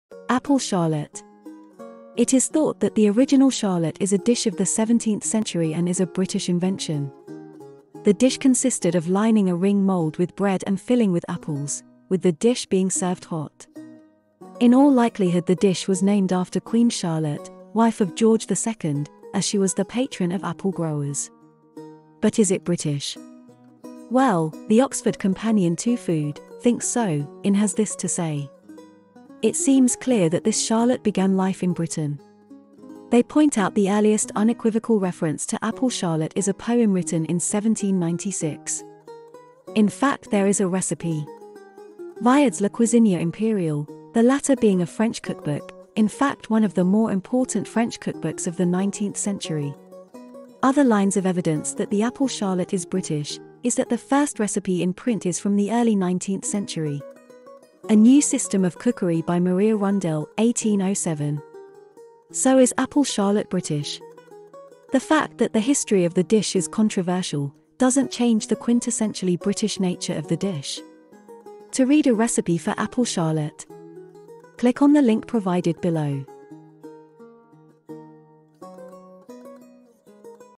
All About Apple Charlotte
Apple Charlotte
The original 'Charlotte' is a dish of the 17th Century and is a British invention. The dish consisted of lining a ring mould with bread and filling with apples, with the dish being served hot. In all likelihood the dish was named after Queen Charlotte, wife of George II, as she was the patron of apple growers